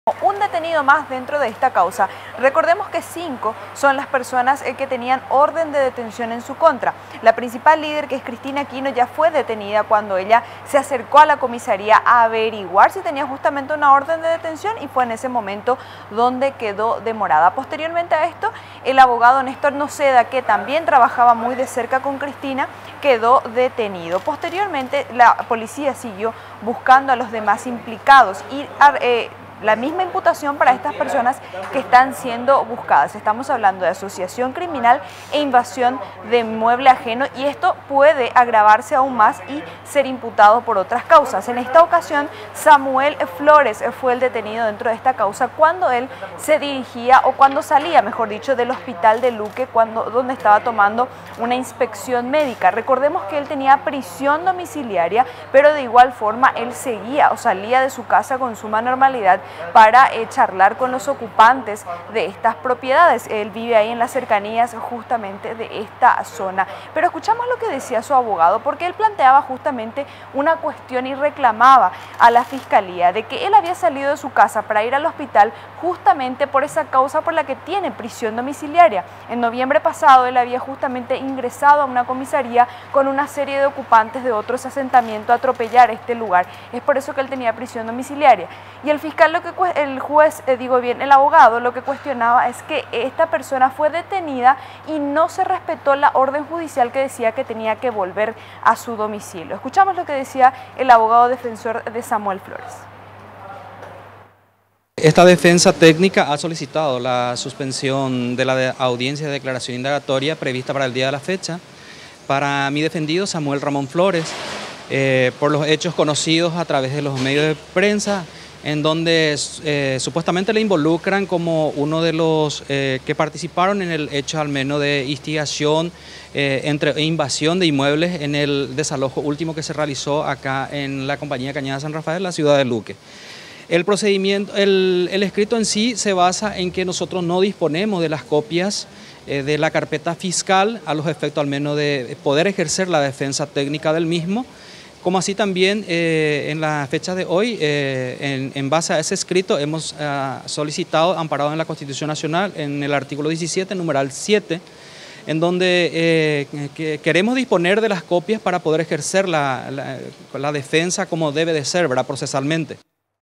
Un detenido más por ocupación
El abogado defensor de Samuel Flores solicito la suspensión de la audiencia de declaración indagatoria de su defendido por instigación de invasión de inmuebles.